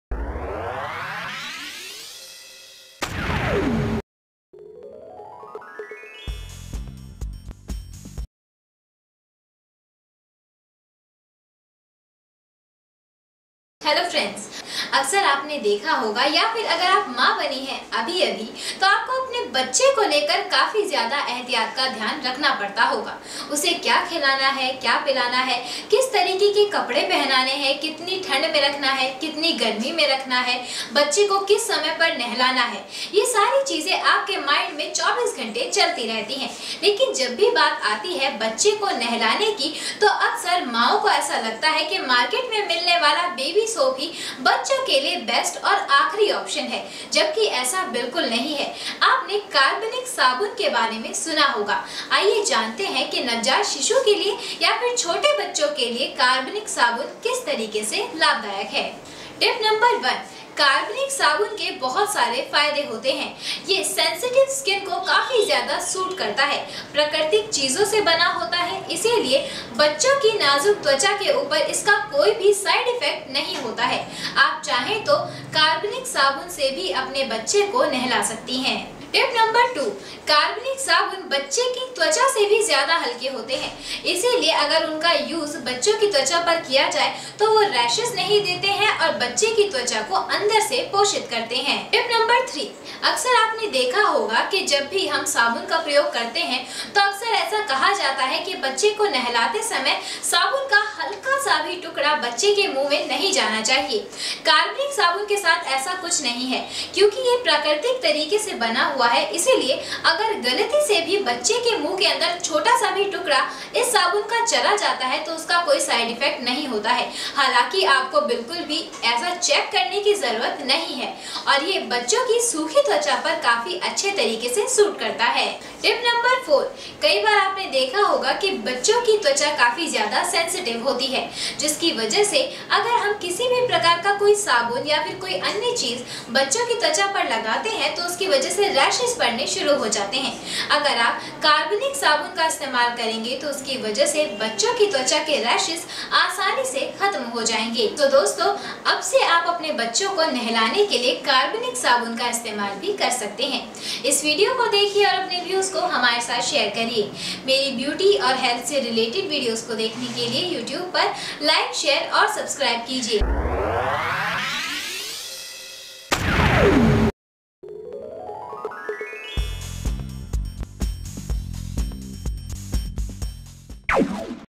जानिए कार्बोलिक साबुन से बच्चों को होने वाले फायदे-Benefits of Carbolic Soap for Babies-SM News
जानिए कार्बोलिक साबुन से बच्चों को होने वाले फायदे